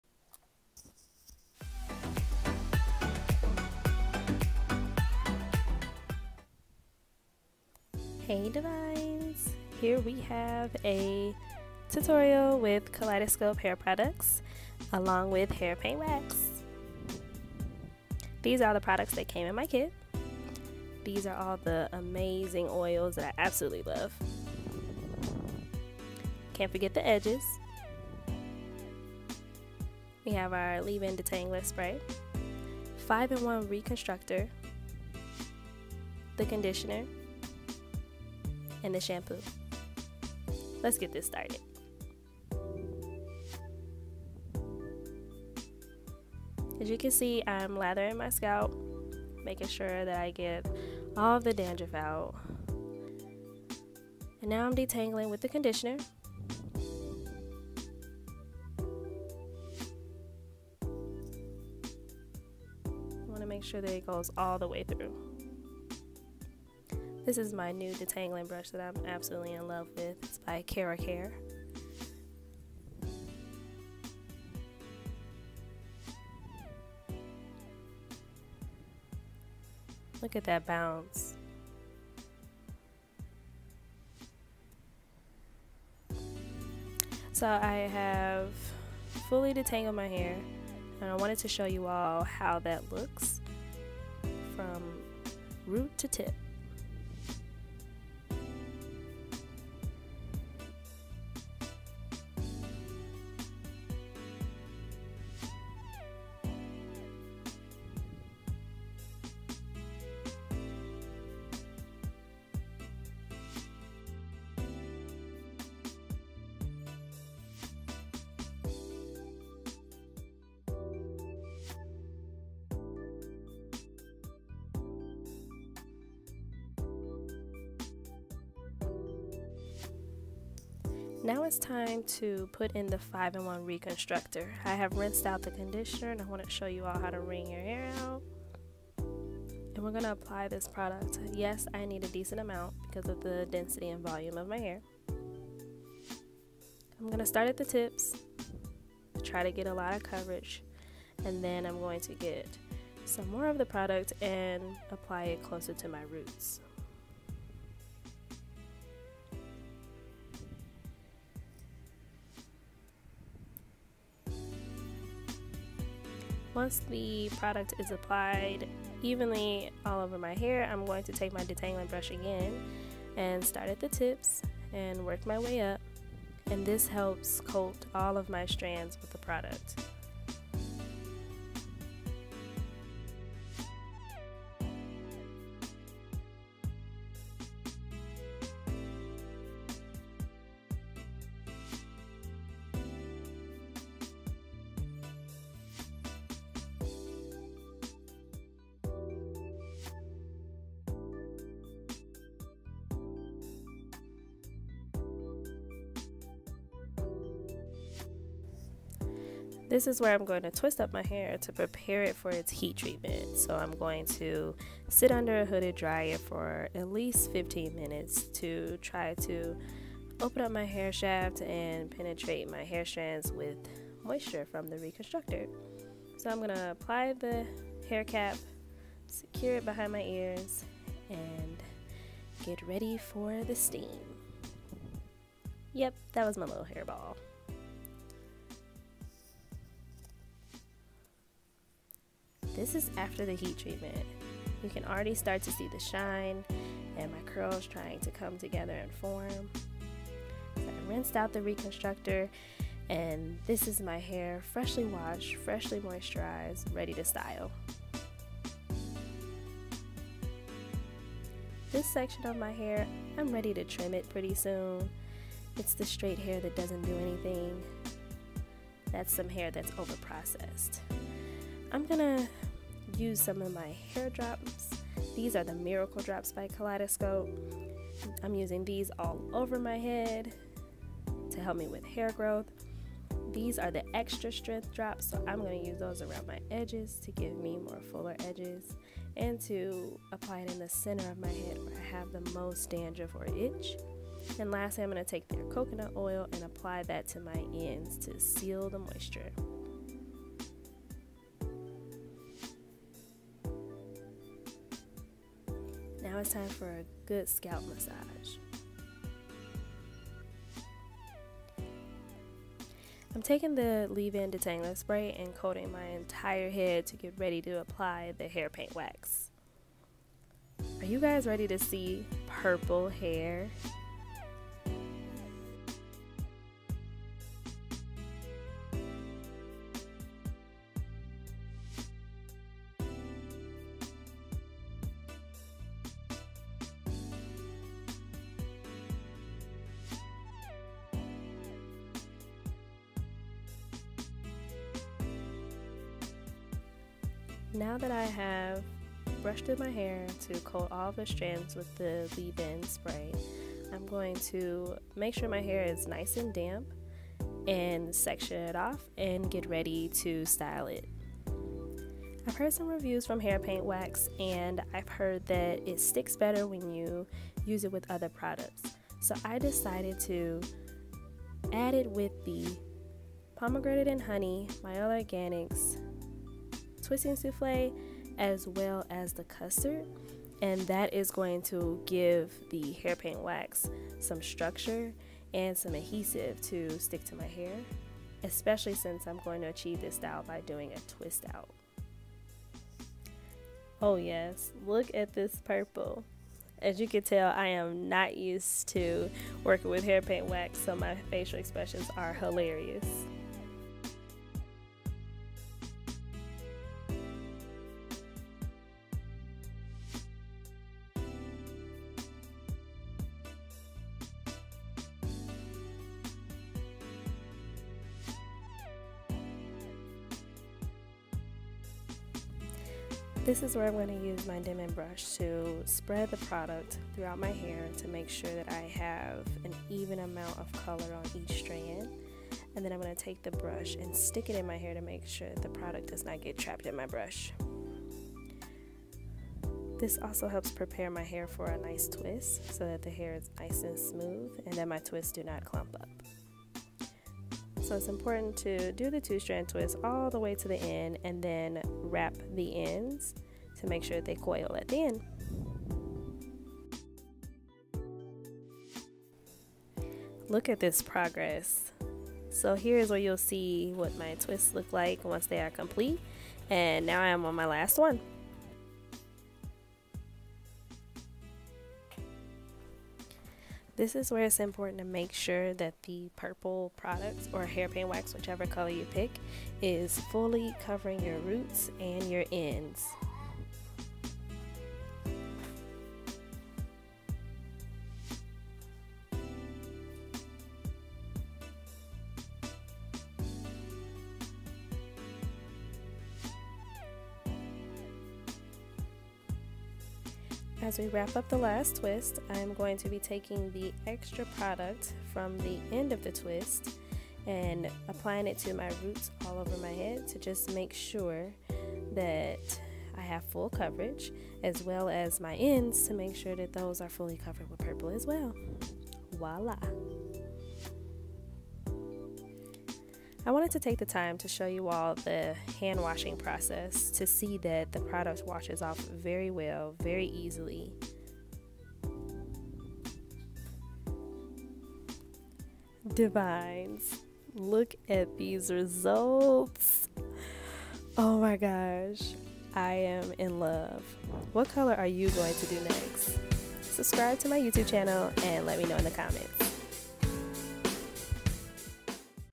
Purple Hair Paint Wax - Full Process
Products Used:
Hair Paint Wax
Purple
Kaleidoscope Hair Products
Moisture Silk Shampoo
Moisture Silk Conditioner
The Answer 5 in 1 Reconstructor
Original Miracle Drops
Extra Strength Miracle Drops
Coconut Miracle Drops
Miracle Edges

♡ Devine Glamour® LLC ♡ Devin Churchill ♡